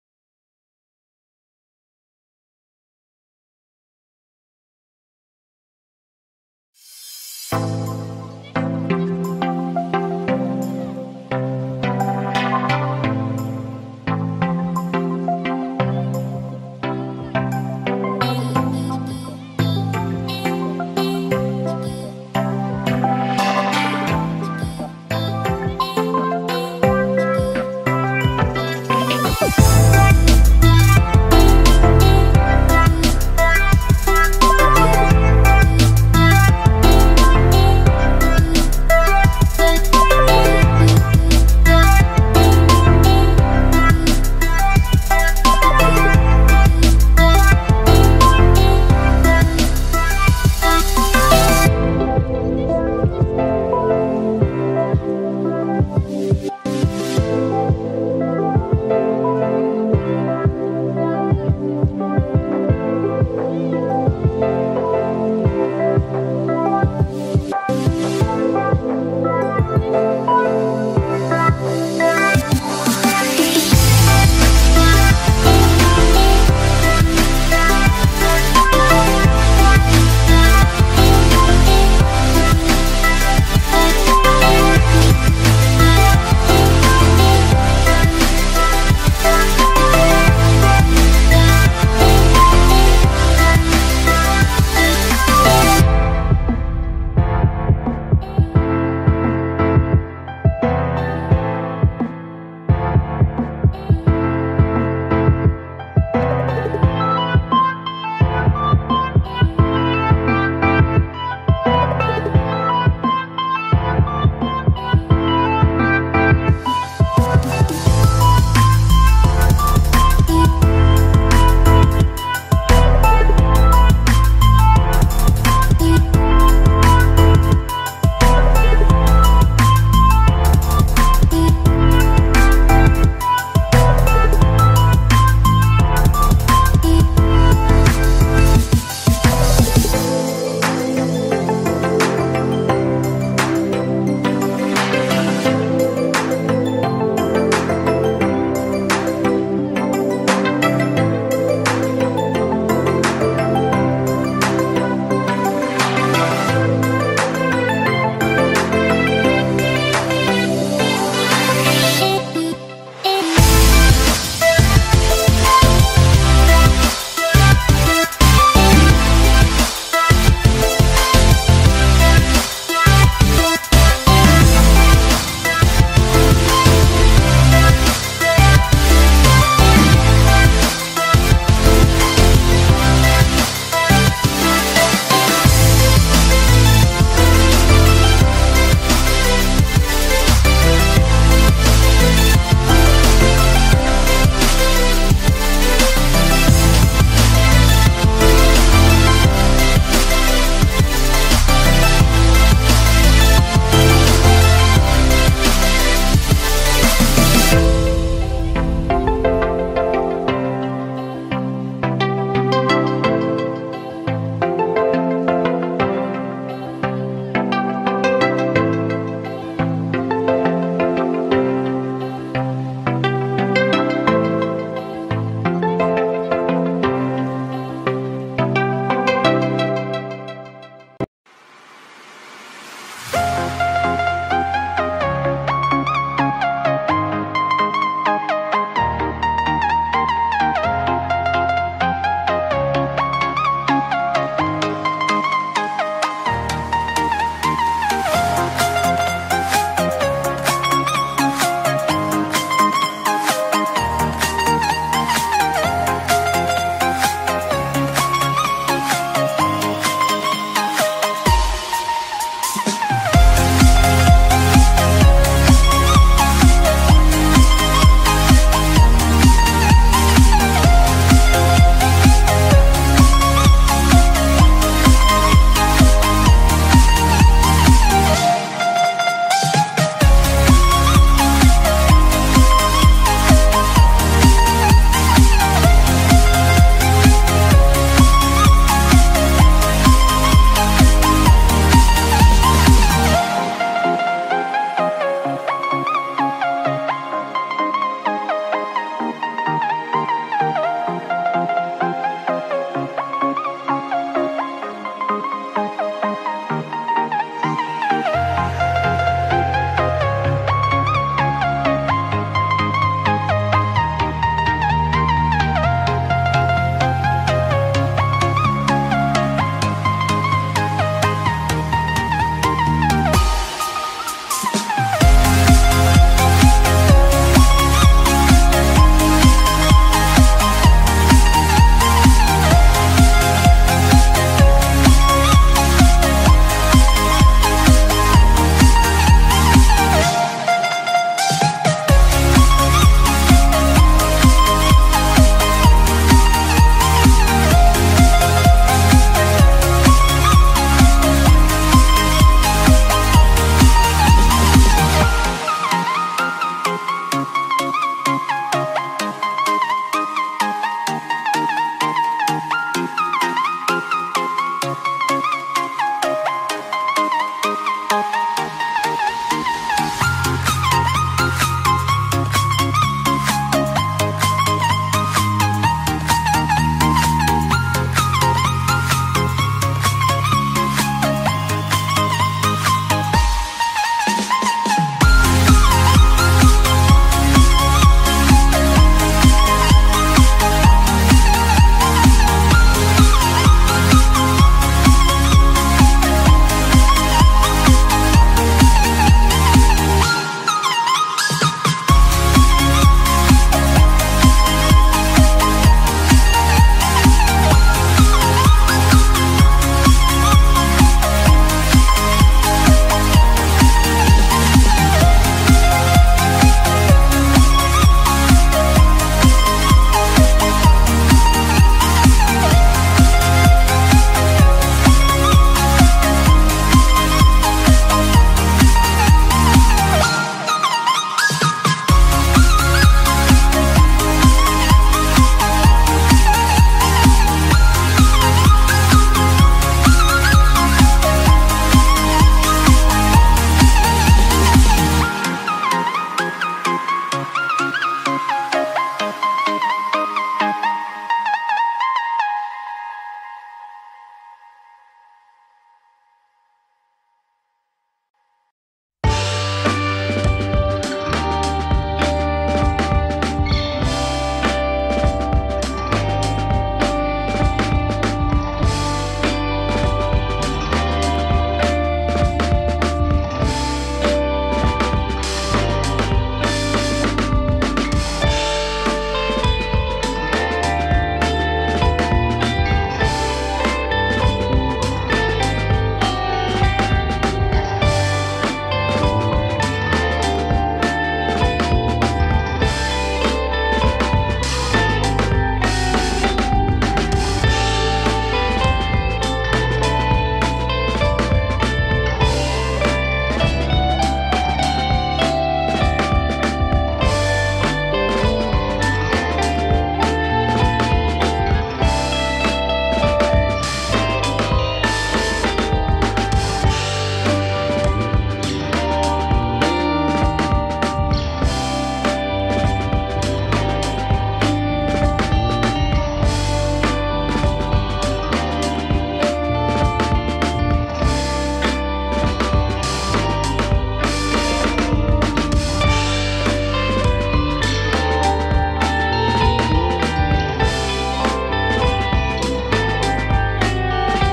GWH 1/48 F-15D Build - Part4 Final
•Detail up
•Weathering
•Assembly
•Finish work